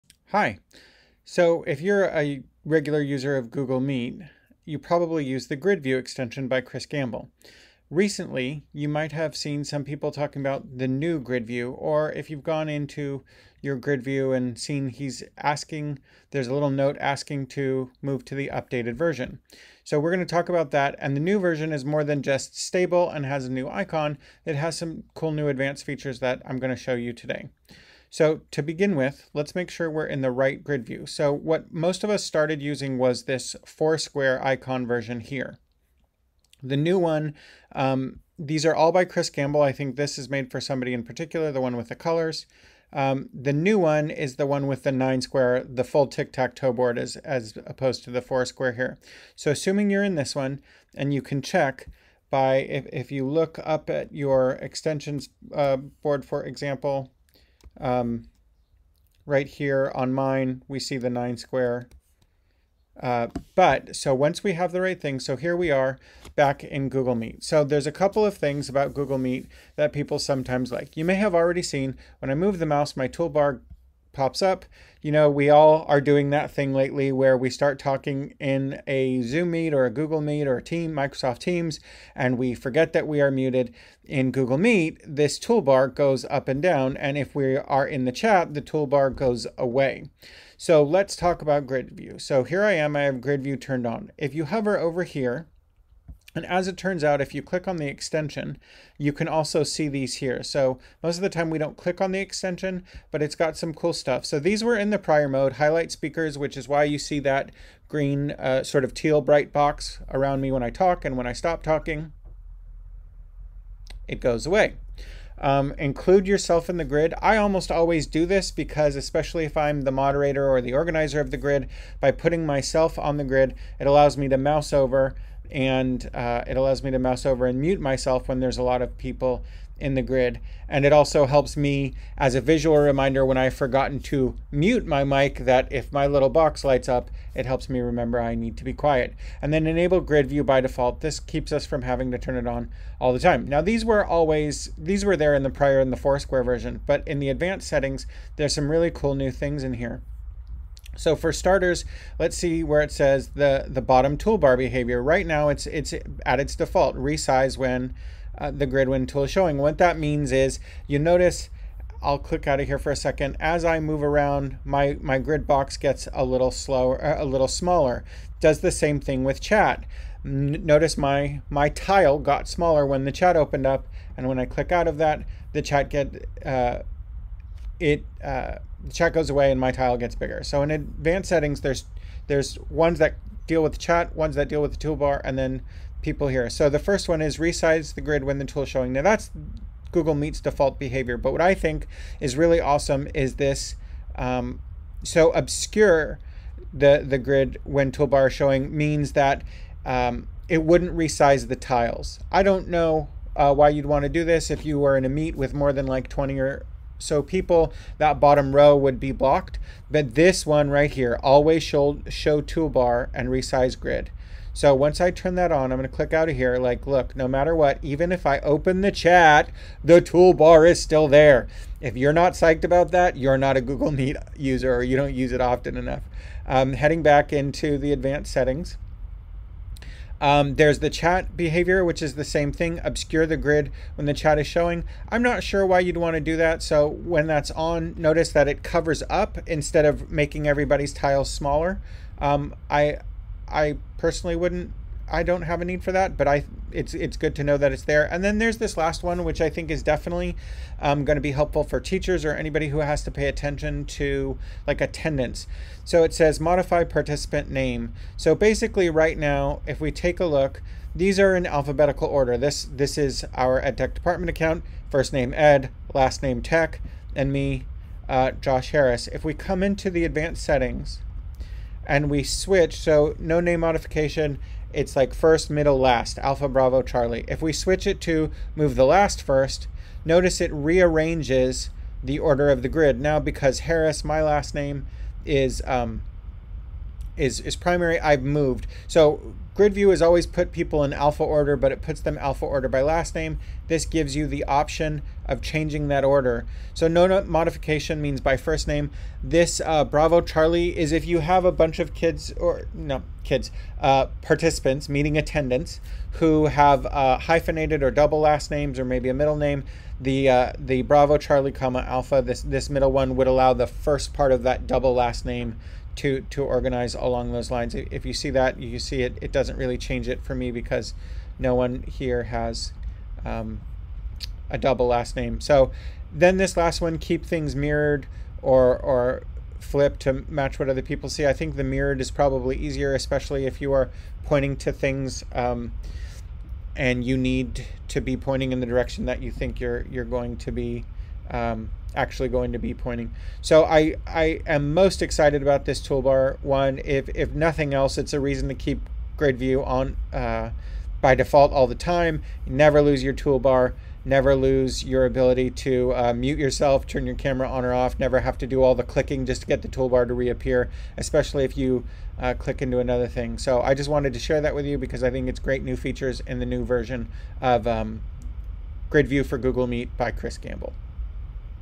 New & Advanced Grid View Features For Google Meet
KEEP YOUR GoogleMeet TOOLBAR ON... ALWAYS!!!
The newest version of Grid View for Google Meet has lots of great advanced and hidden features that can make Google meet even more of a pleasure to use.